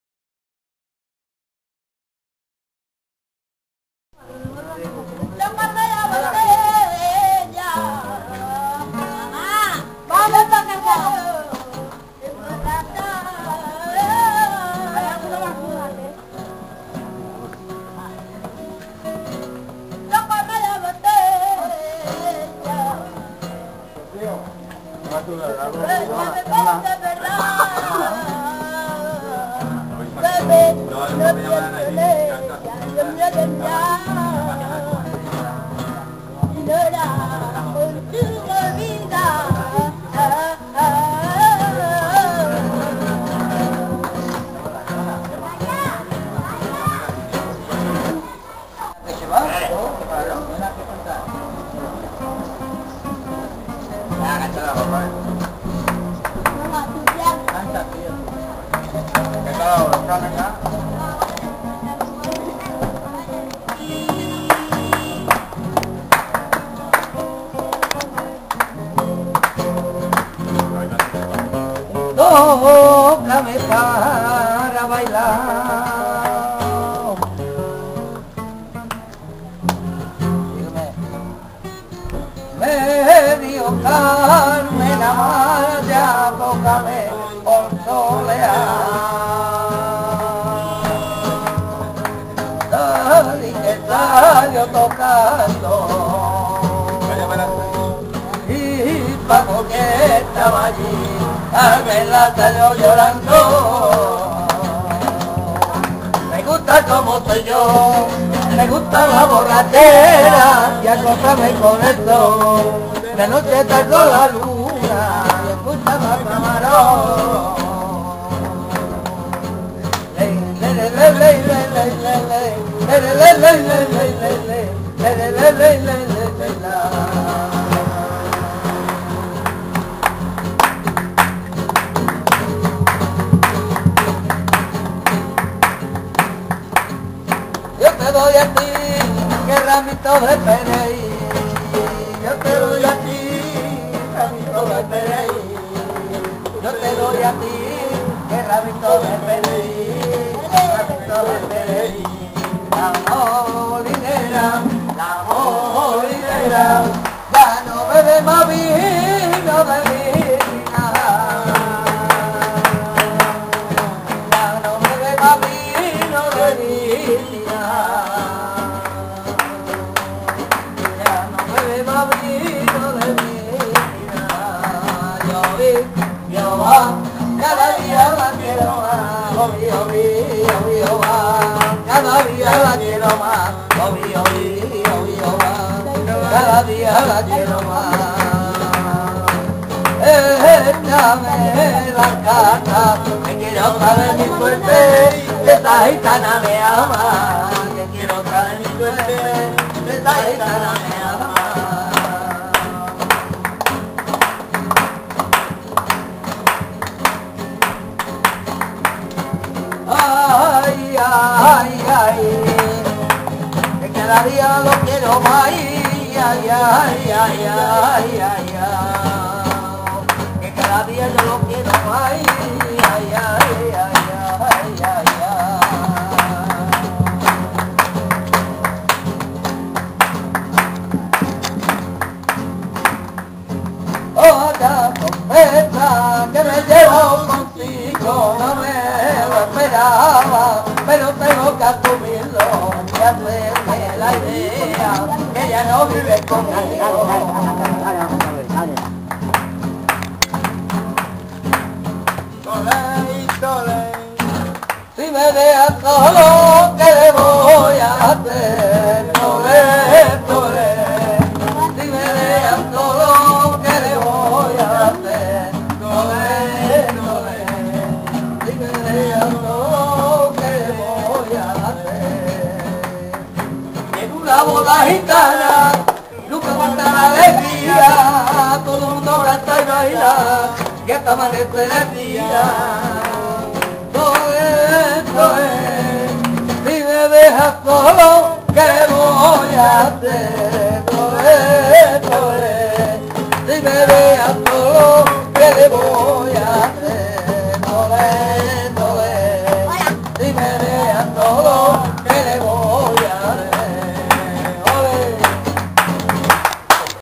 El Chocolate canta
El Chocolate niño de 11 años nos canta flamenco en el Cinto
Supervisa Los coros El Camarón